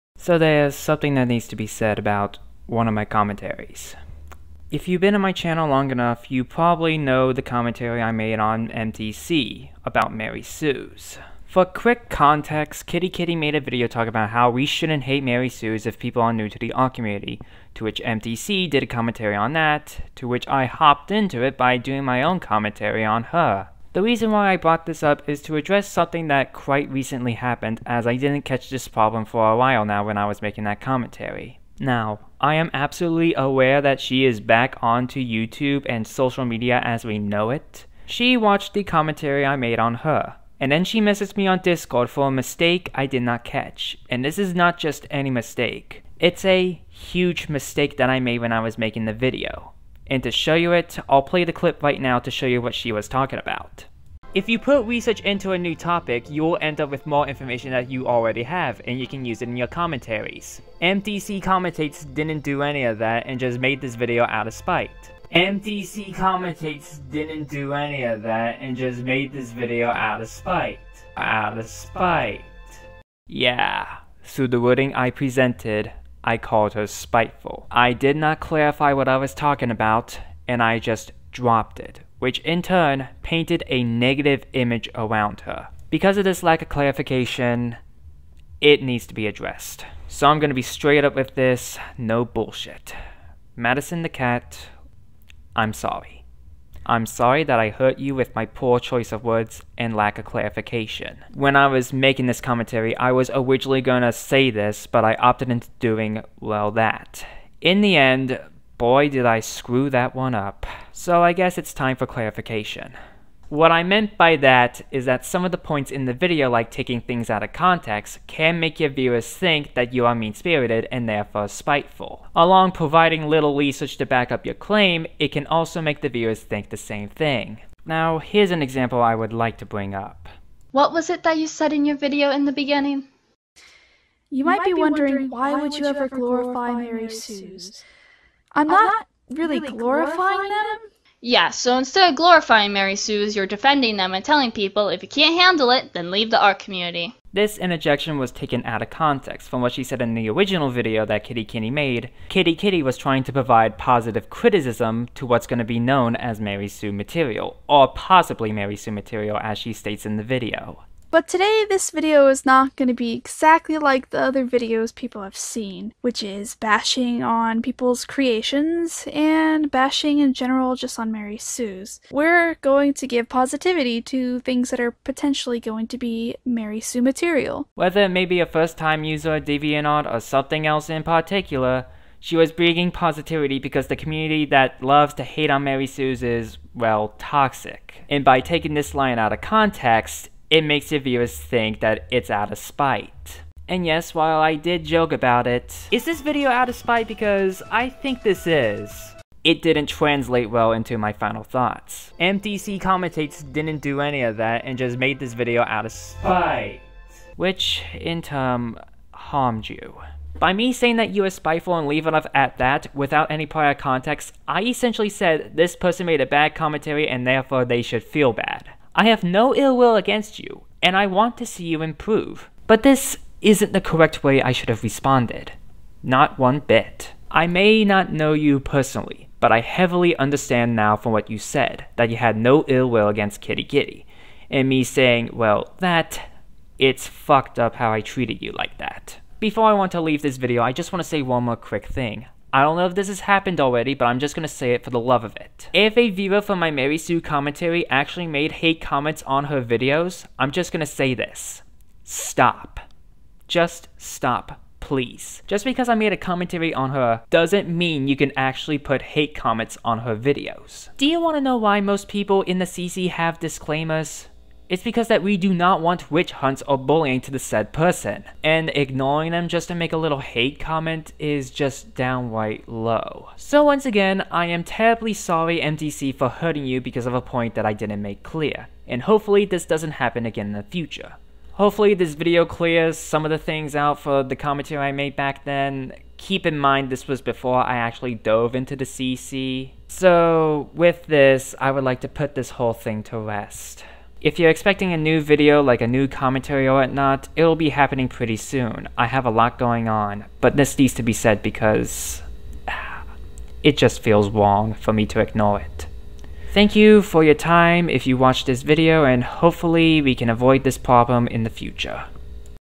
Apology to MTC-Commentates - Clarifying a not so explained point
Before I dove into the CC with my failed comm on Doodle, this was a commentary I did back then, while I still do stand by my points back then, I screwed up on one point which caused harm.